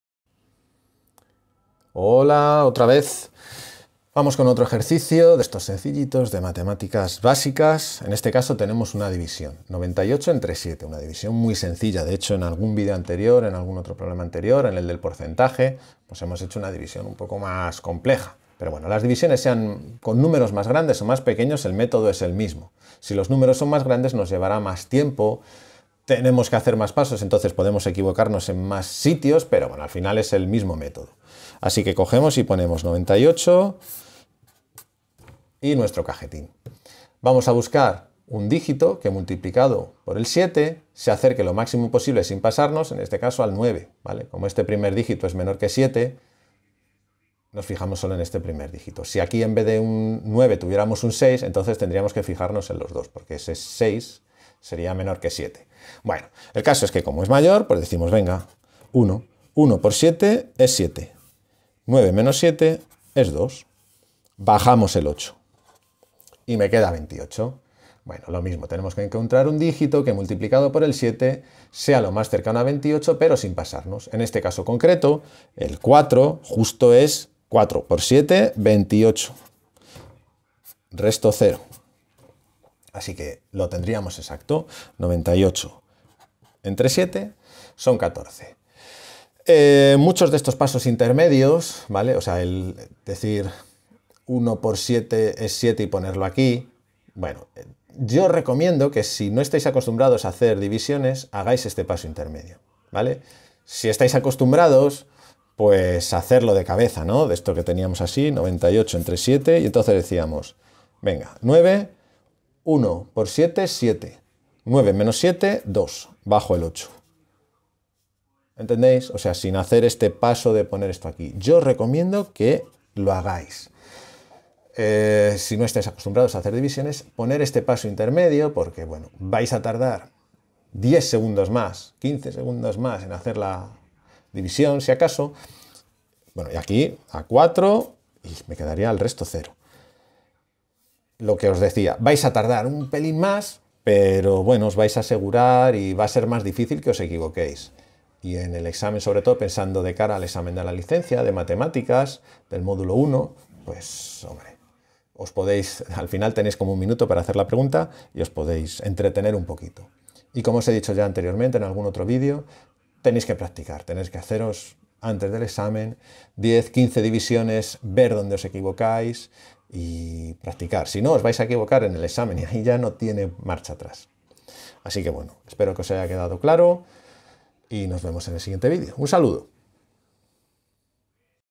EASA. M1. División (7/26)
Conocimientos matemáticos básicos para el técnico de mantenimiento de aeronaves (mecánico y aviónico). En este vídeo explico cómo realizar una división sencilla.